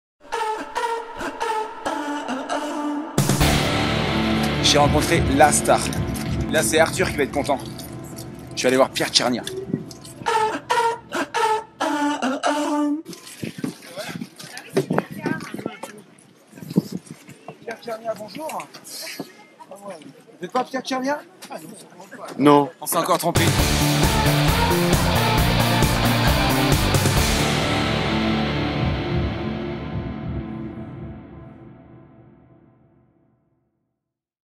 05 : Pierre Tchernia des Enfants de la télé
Frank Cotty rencontre des stars... ou presque ! Mais ces derniers ne le savent pas toujours... Frank a du culot, de l'humour, il est provoc', osé, odieux, sans pitié ni scrupule ! Certains le prendront avec humour, d'autres non...